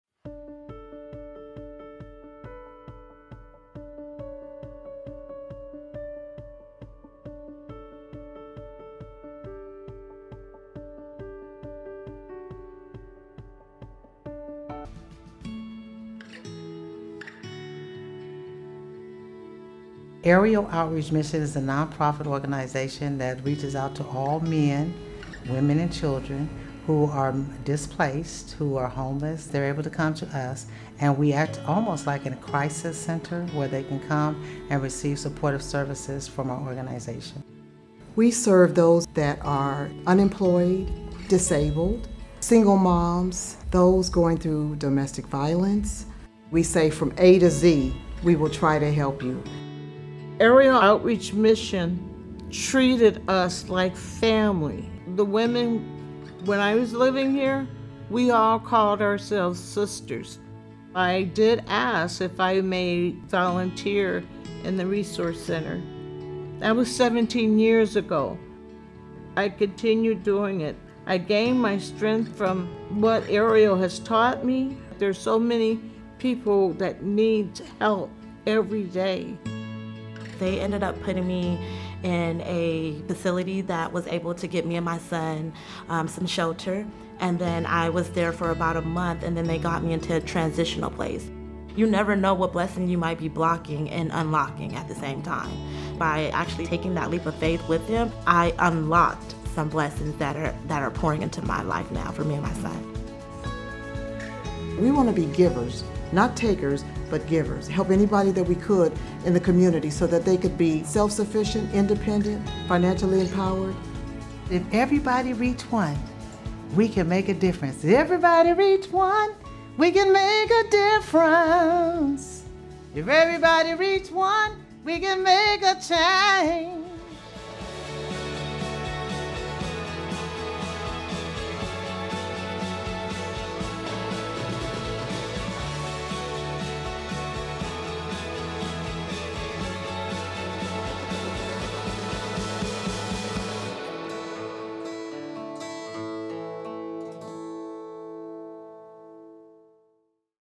[Episcopal Impact Fund] Grantee Spotlight: Ariel Outreach Mission
Ariel Outreach Mission provides temporary housing and support services to women and their children in crisis, as well as other grassroots support services for very low-income residents of East Oakland and the East Bay.